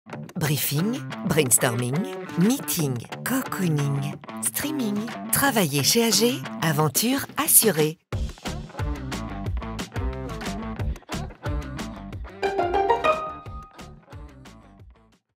Découvrez vos otpions chez AG
Chez AG, l'innovation et le développement personnel occupent une place centrale. Qu'il s'agisse d'échanger des connaissances avec des collègues ou de collaborer à des projets ambitieux, chaque jour offre de nouvelles possibilités d'apprentissage et de développement. Ensemble, nous construisons l'avenir, petit à petit.